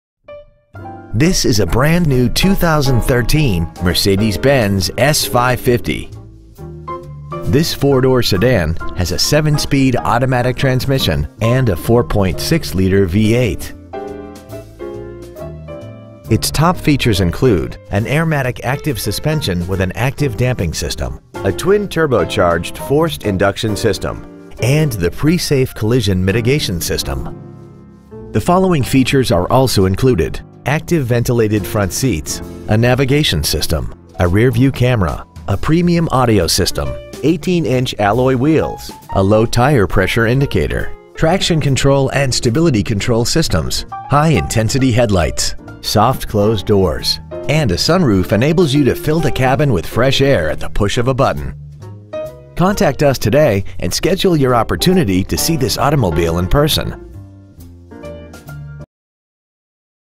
2013 Mercedes-Benz S550 Houston TX 77090
We are proud to present this 2013 Mercedes-Benz S550 .

 Year : 2013
 Make : Mercedes-Benz
 Model : S550
 Engine : Gas V8 4.6L/285
 Trans . : Automatic
 Exterior : Black
 Miles : 5
 Interior : BLACK